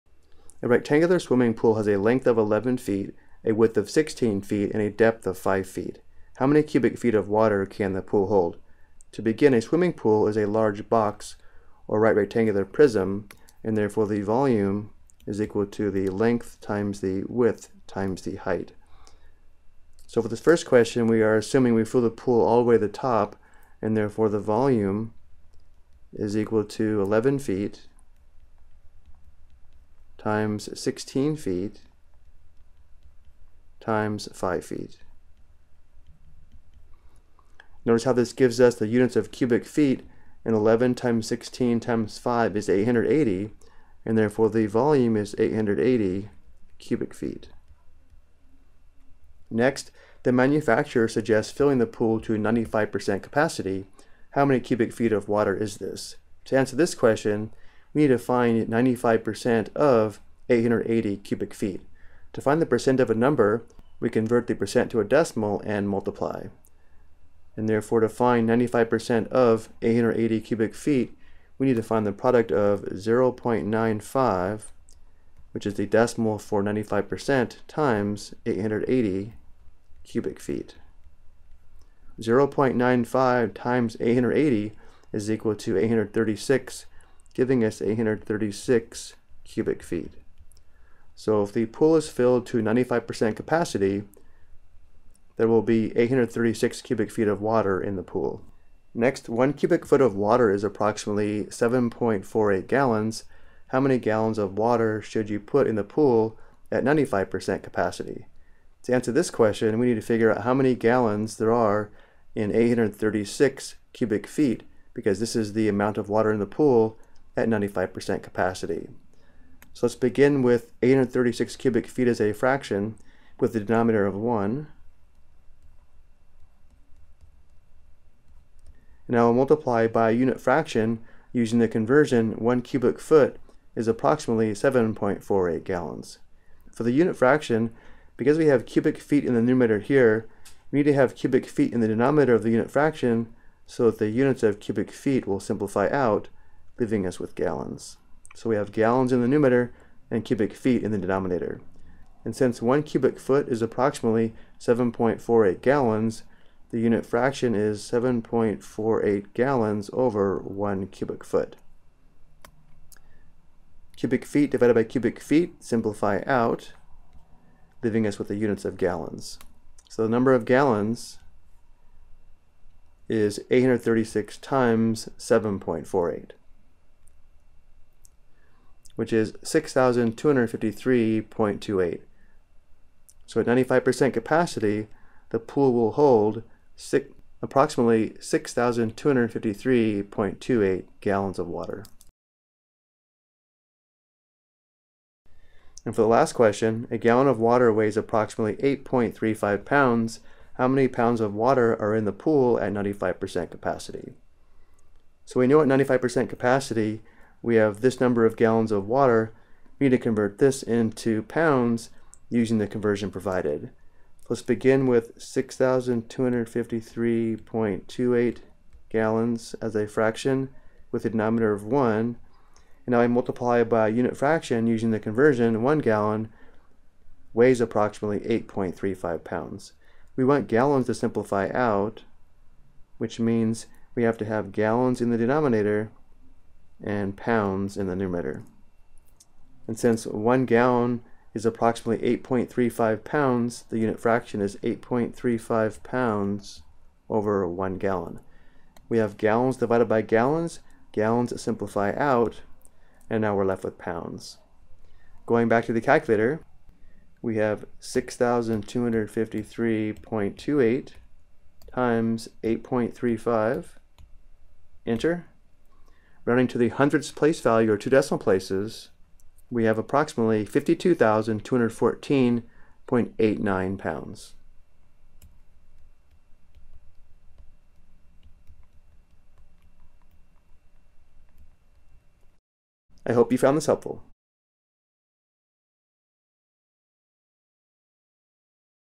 Conversion App: Determine Volume, Gallons and Weight of Water in a Pool
This video explains how to determine a volume of a pool, the number of gallons of water in a pool, and the weight of the water in pounds.